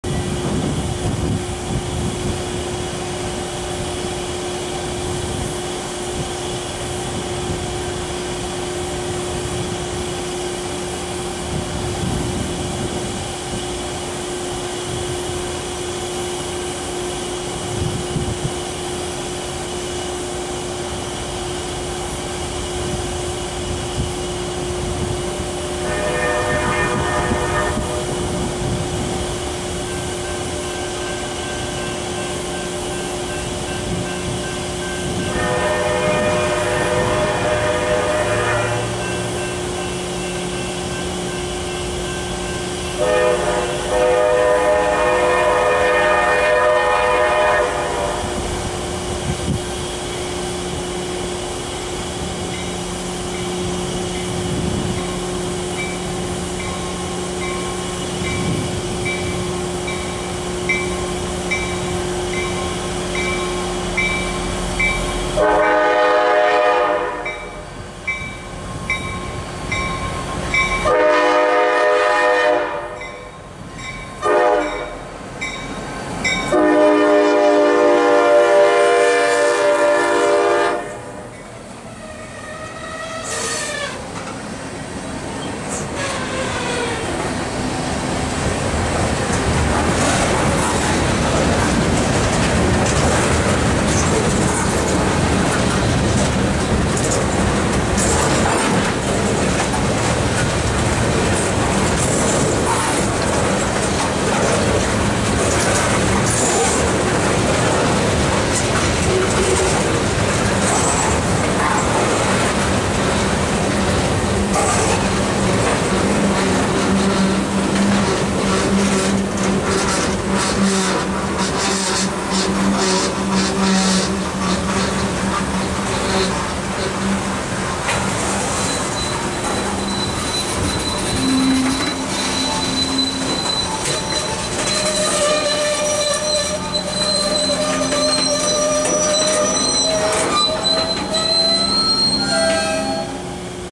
CSX GP40-2 6483 w/ Nice K5LA Leads Local Y126-12 on 2/12/22
Two CSX Geeps on job Y126-12 I missed go south earlier from P&G now return north on Track 2 with a cut of tankers and a hopper car. Of course Q361-11 was coming south on Track 1 and Y126 decided to stop in front of me, so I had to rush down to the next crossing to get a decent shot of Q361. Strangely, 6483 was on Tullahoma local M780 back in October, and now I find it up here four months later.
Filmed at Kings Run Dr. in St. Bernard, OH on the Cincinnati Terminal Sub.

CSX 6483 (ex SCL 6621)- GP40-2 (Hybrid Raised Letter K5LA)
CSX 2677 (ex L&N 6026)- GP38-2